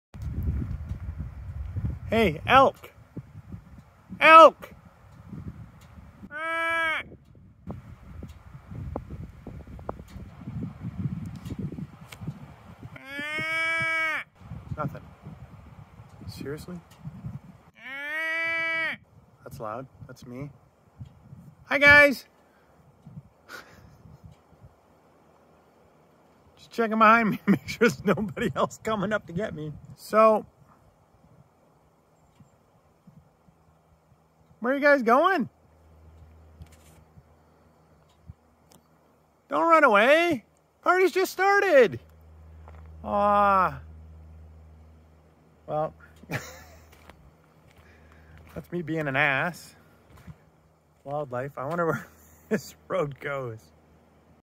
Elk not Impressed
While out exploring in my Jeep, I saw some tracks in the snow. I got out to investigate and saw a small herd of Elk.

I tried to grab their attention by calling but they did not seem interested. When I turned around to make sure something wasn't coming up behind me, that movement seemed to be what bothered the Elk.

The Elk then walked into the bushes and out of sight.